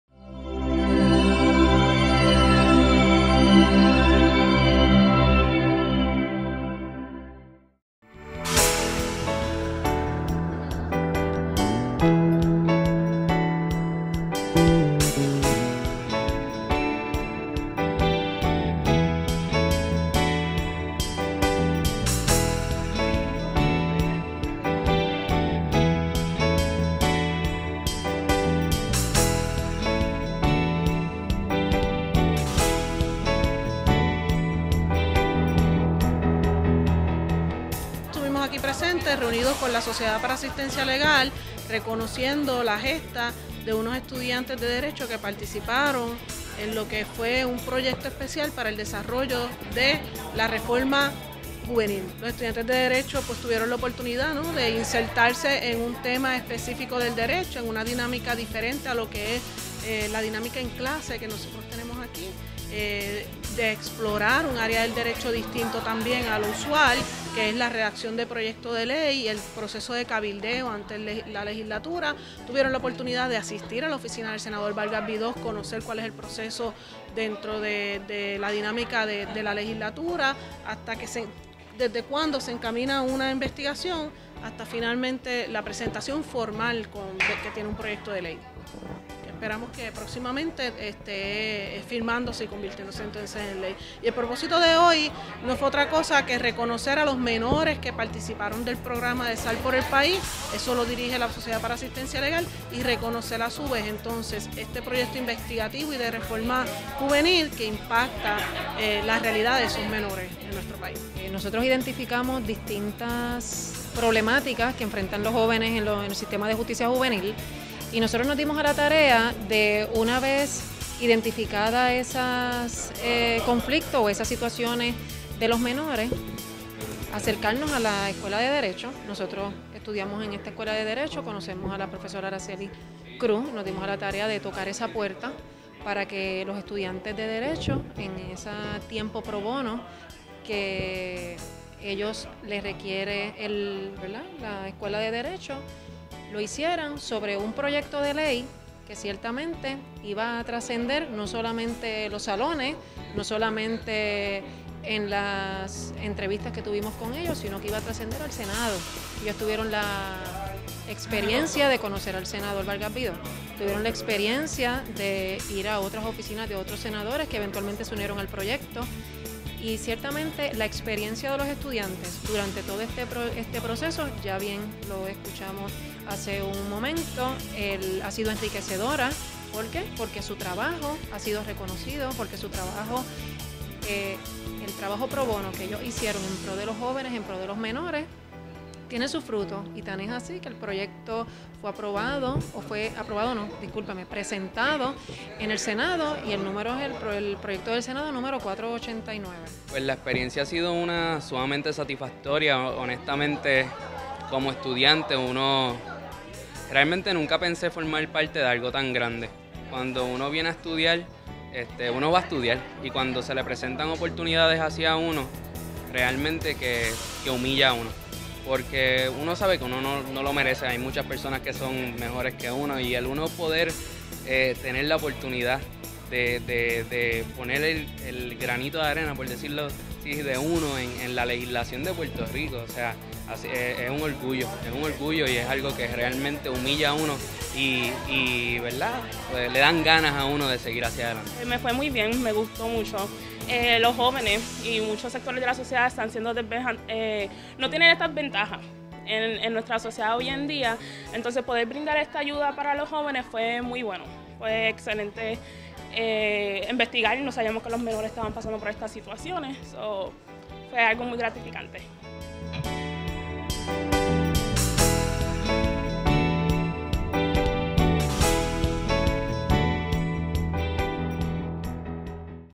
Reconocen labor protagónica a estudiantes de derecho
Diez estudiantes del Programa de Pro Bono de la Escuela de Derecho de la Pontificia Universidad Católica de Puerto Rico (PUCPR) fueron reconocidos por su excelente colaboración en la redacción, investigación y presentación del proyecto de ley que se conocerá como “Reforma de Justicia Juvenil” (P. del S. 489) . Los estudiantes colaboraron voluntariamente con la Sociedad para la Asistencia Legal  como parte del Programa Pro Bono Estudiantil de la Escuela de Derecho. El Programa desarrolla y colabora en  proyectos en los que los estudiantes se destaquen al realizar labor social-comunitaria orientada al derecho.